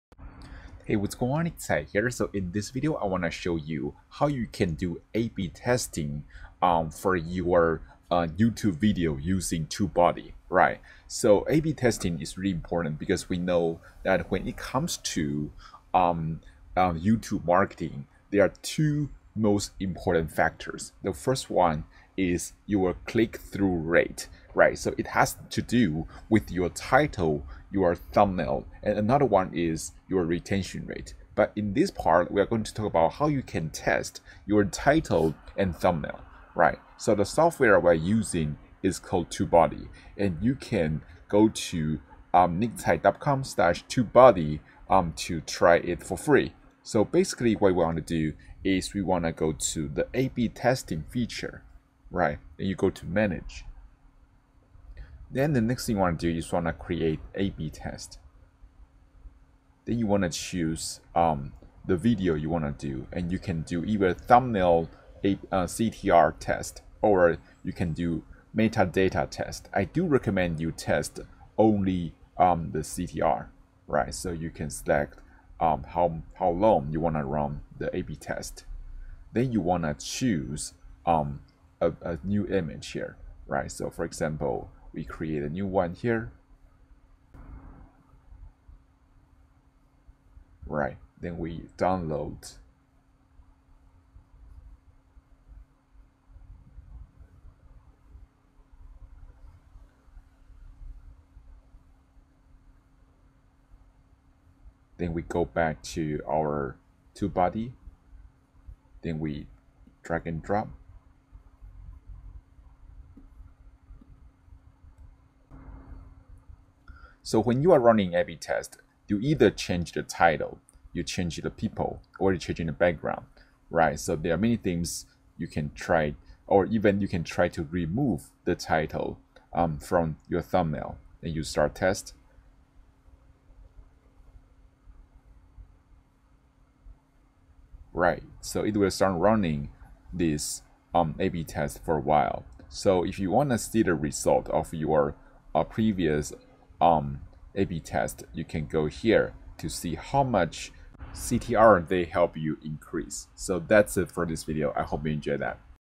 How to Run Ab Testing for YouTube Video With Tube-buddy
Want to get more traffic and turn your following into your customers? Sign up for my free training here